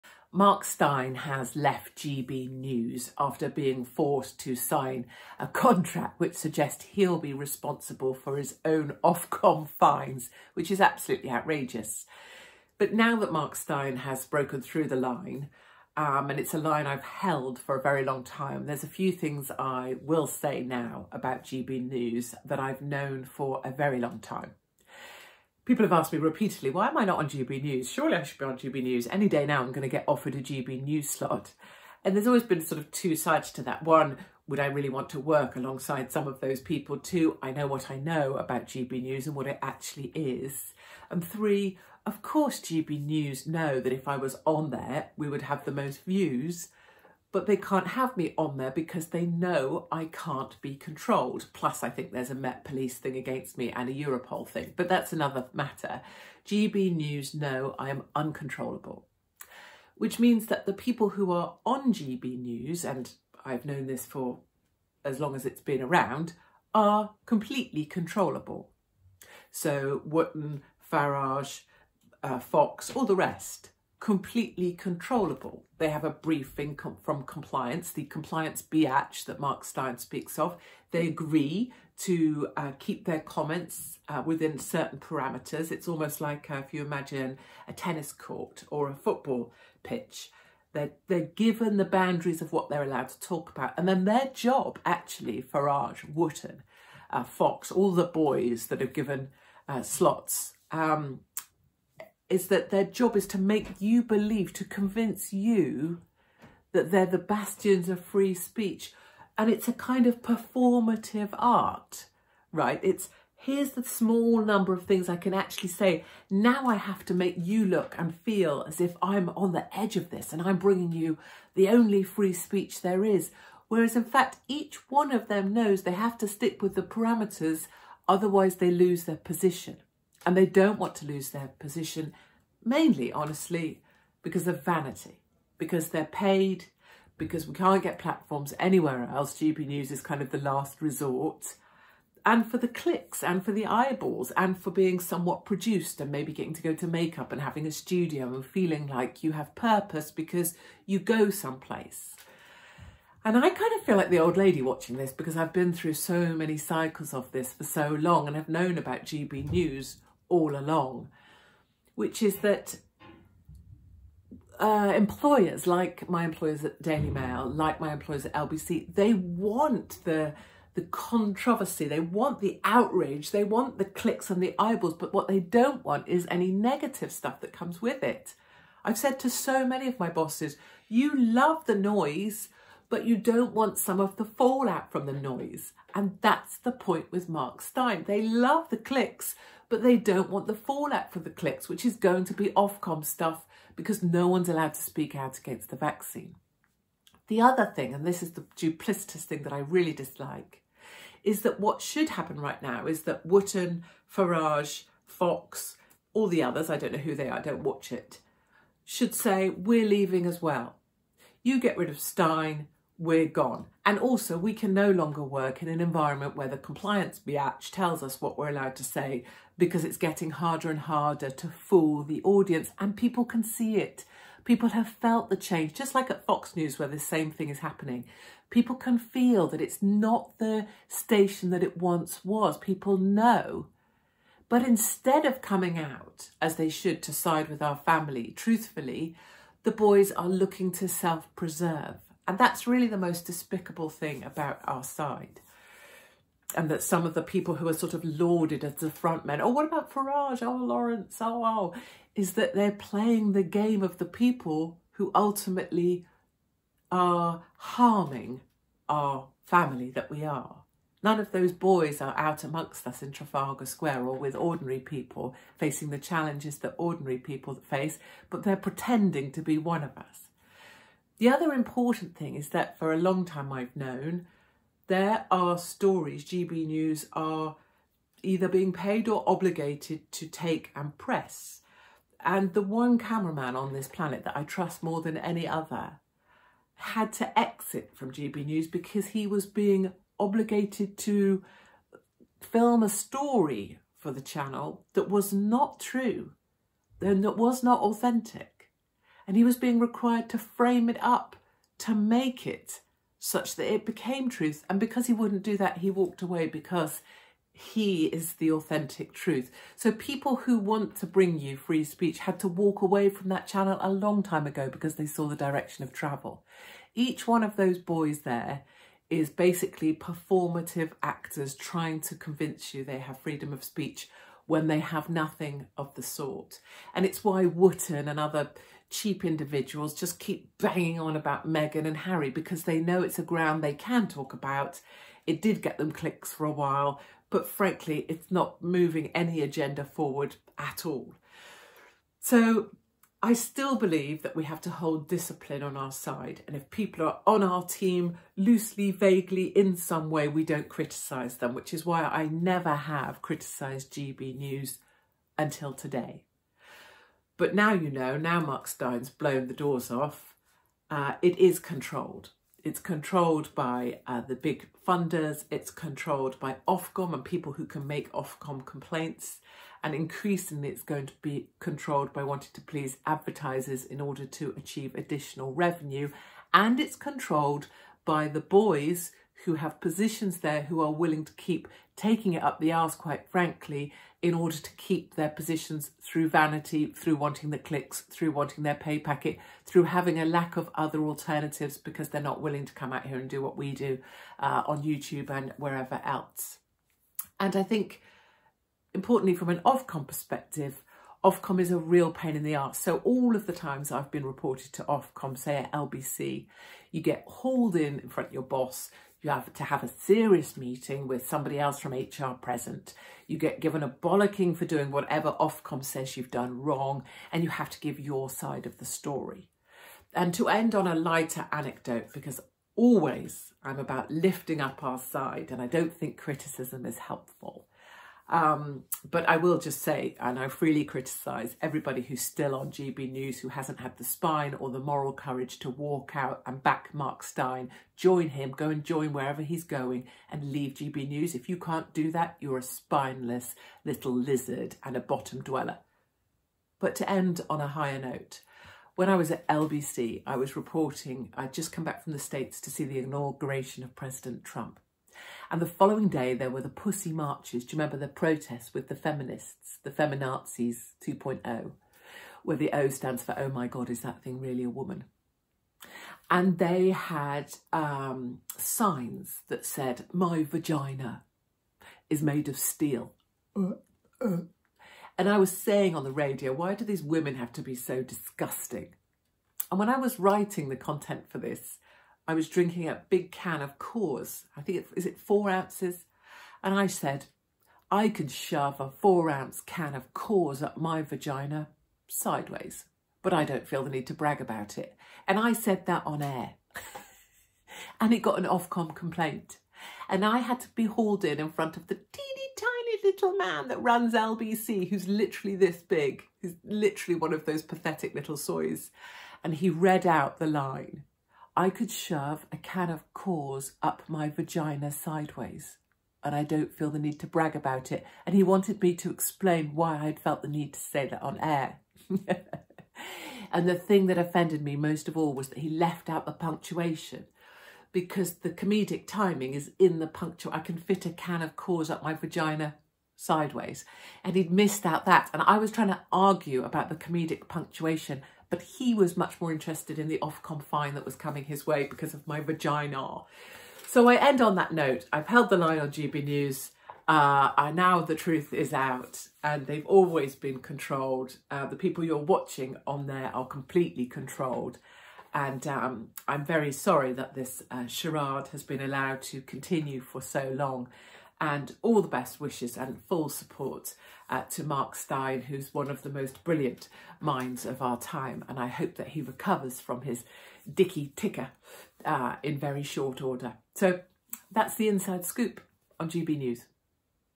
Katie Hopkins: The truth about GB News -- I have been keeping this to myself...until now.
Katie Hopkins on...